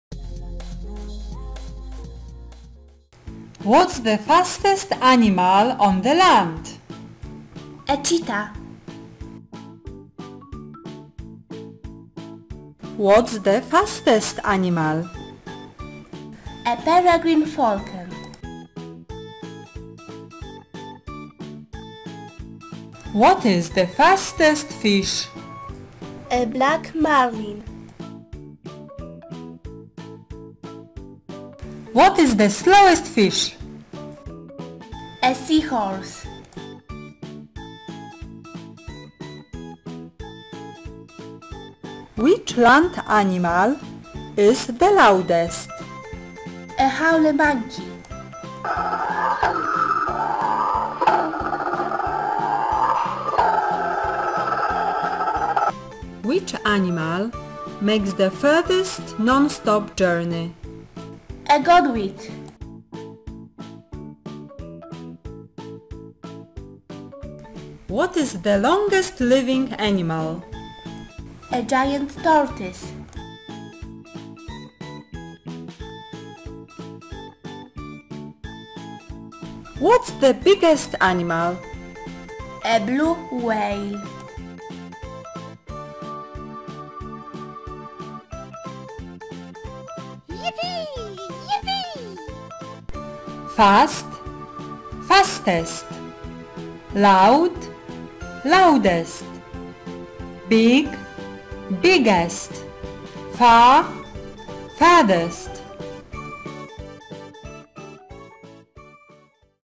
My English Lekcja 29 Quiz Zwierzęta-rekordy (the biggest, the fastest, the loudest)
Rekordy wśród zwierząt (the biggest, the loudest)